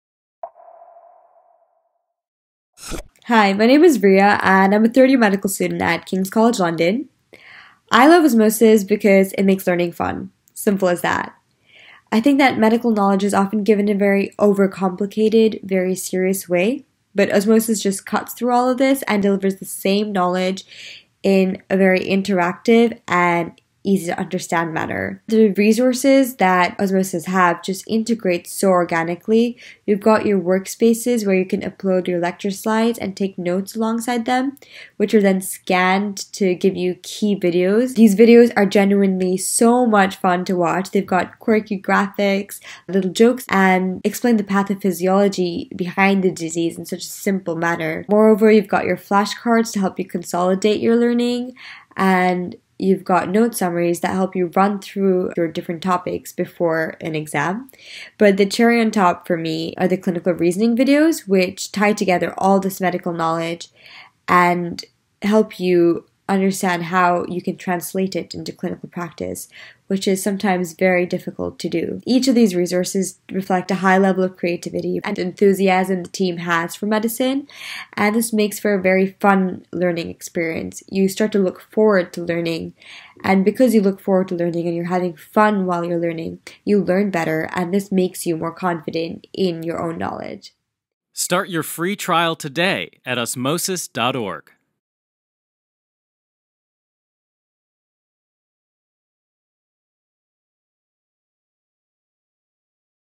Osmosis.org at King's College London
Riya from King's College London in the UK talk about their Osmosis experience! Find our full video library only on Osmosis